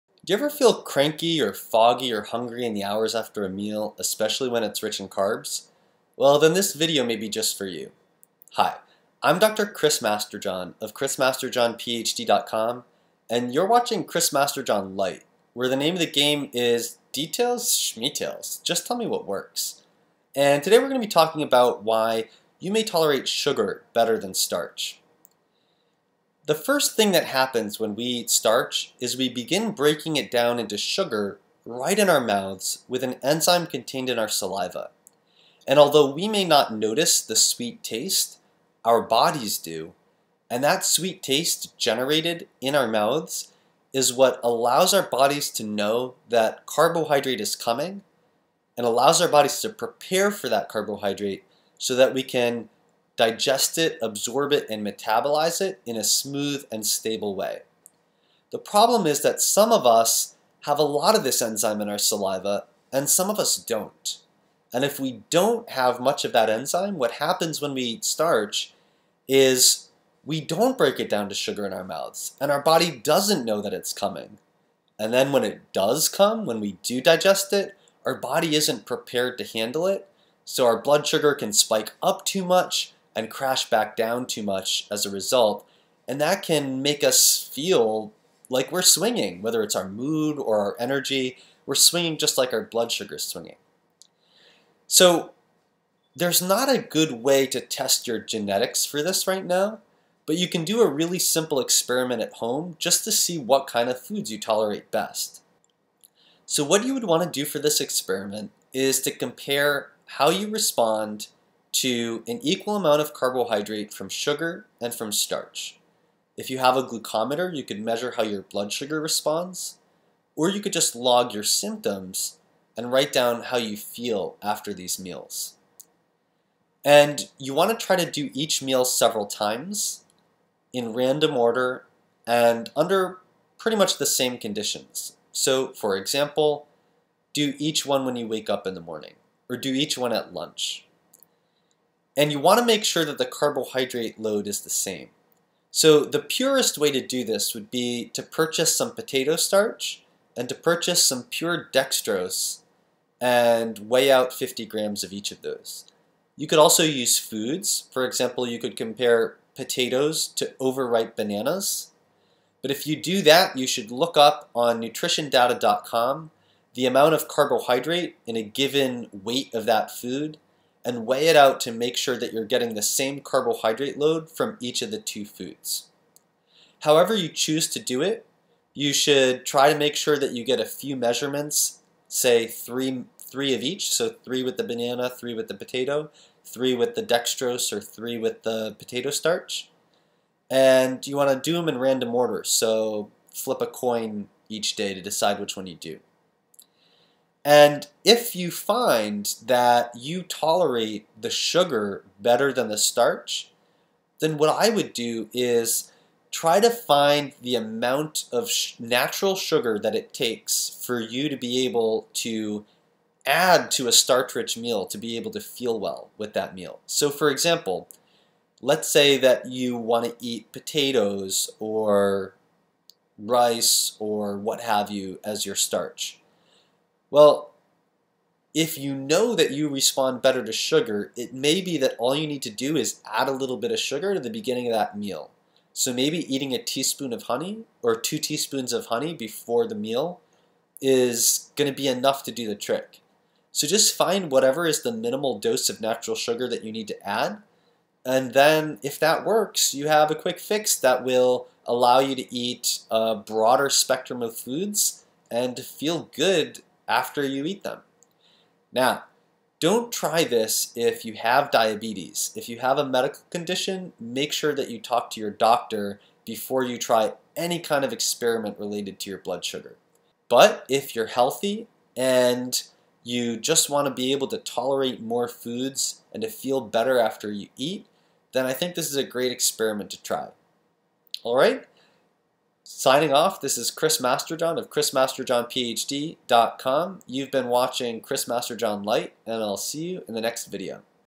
Sometimes Sugar Is Better Than Starch
Chris Masterjohn, PhD is the Founder and Scientific Director of the mitochondrial test mito.me. Although starch digests more slowly than some sugars and starch-rich foods often contain fibers that slow the release of glucose into the blood even further, the presence of glucose in the mouth helps our bodies coordinate the proper insulin response needed to keep our blood sugar stable. Some of us may tolerate natural sugars better than starches because we have low amounts of the enzyme salivary amylase, which begins converting the starch to sugar within our mouths. If your blood sugar response to starch is poor, providing you do not have diabetes, it is a worthwhile experiment to see if you tolerate natural sugars better.